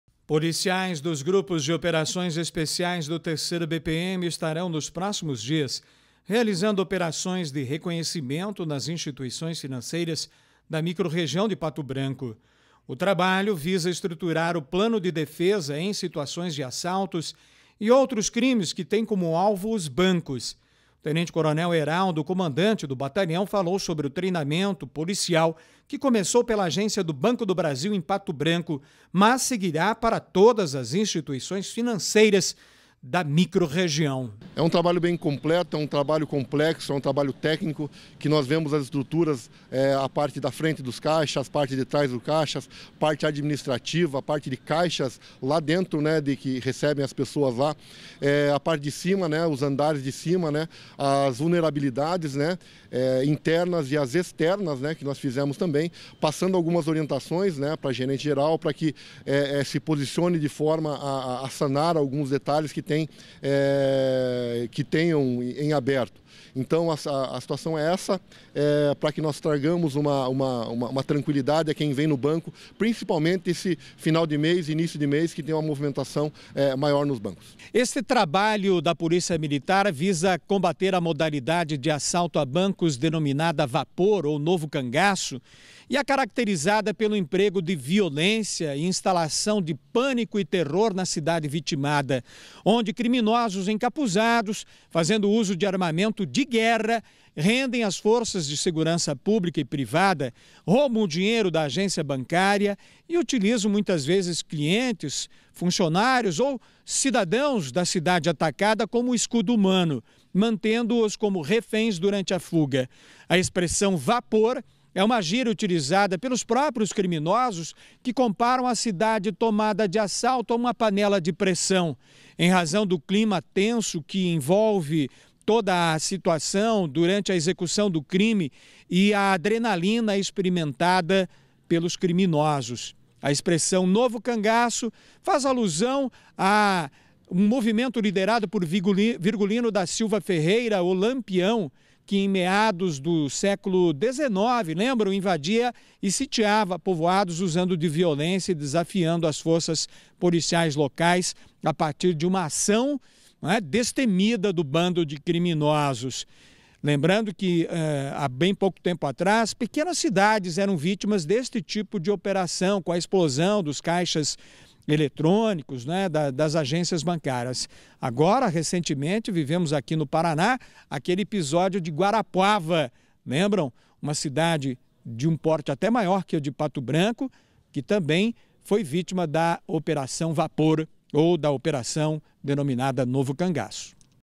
Plano de defesa contra assaltos a bancos
Segurança: Plano de defesa contra assalto a bancos na ação conhecida como “Novo Cangaço” começa por Pato Branco.